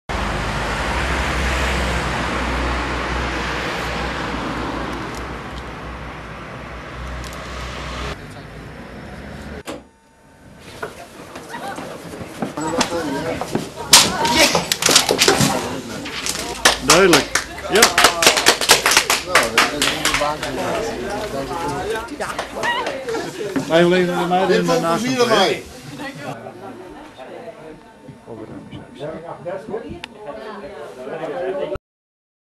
0pruimingsstunt kooistra damwoude
Kooistra geeft gratis een bankstel weg en 2 met hoge kortingen na een nacht waken.